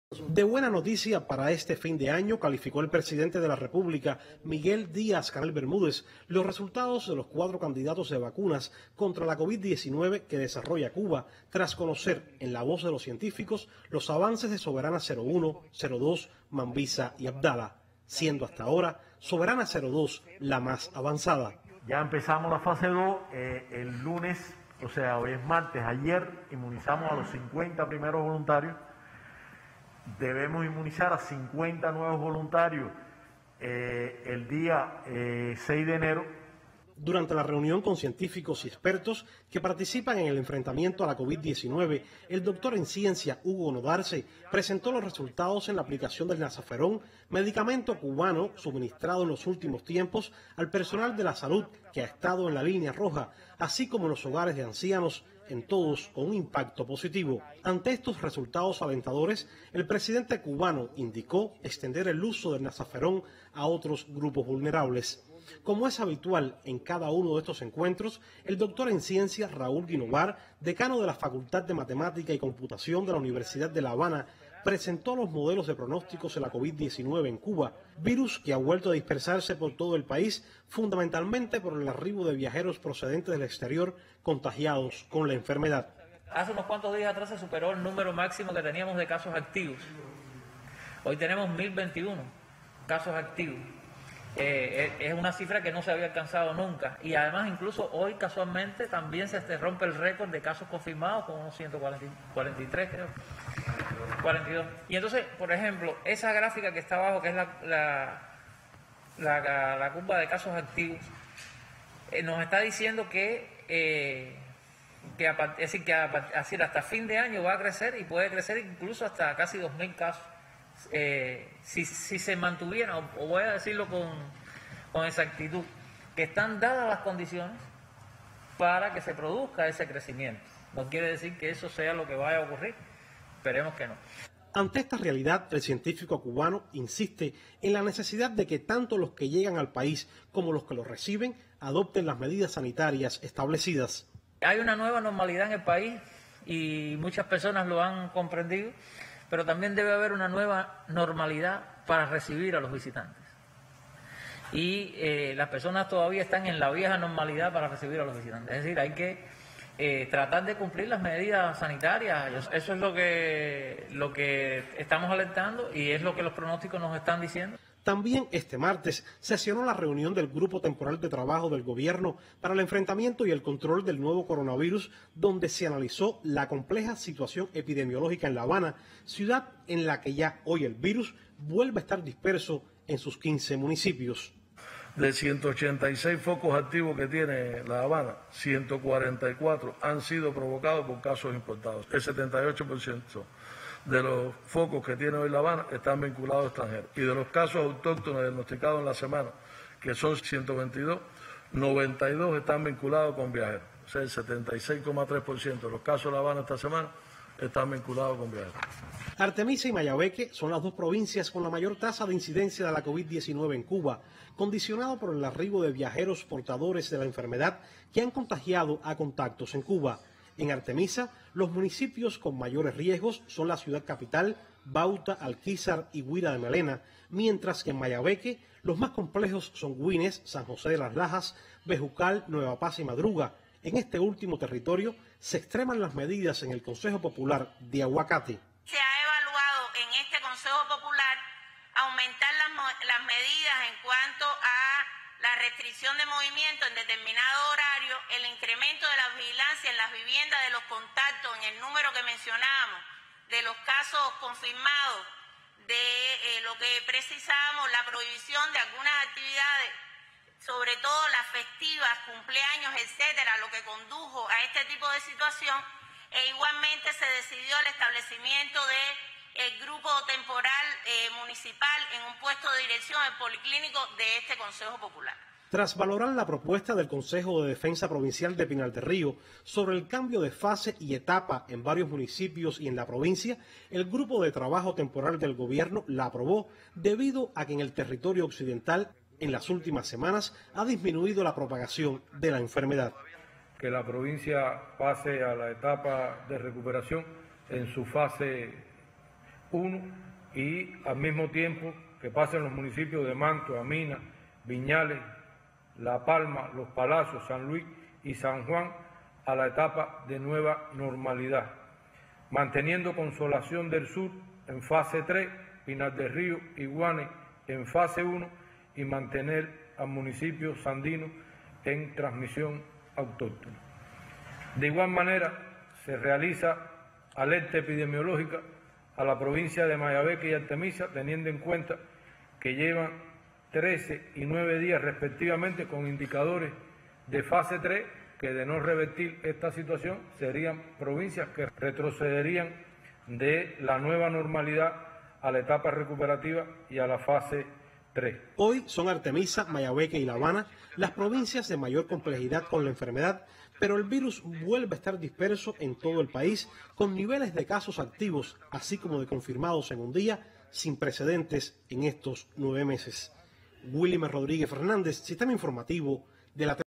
Intercambia Miguel Díaz-Canel con científicos que enfrentan la Covid-19 en Cuba
El Presidente de la República, Miguel Díaz-Canel Bermúdez, sostuvo este martes un encuentro con científicos y expertos que participan en el enfrentamiento a la Covid-19, donde fue actualizado sobre los avances de los cuatro candidatos vacúnales cubanos. Luego, el jefe de estado encabezó la reunión del Grupo de Trabajo Temporal para la prevención y el control del nuevo coronavirus. Ambas reuniones, estuvieron dirigidas por el Viceprimer Ministro doctor Roberto Morales Ojeda. (Sistema Informativo de la Televisión Cubana)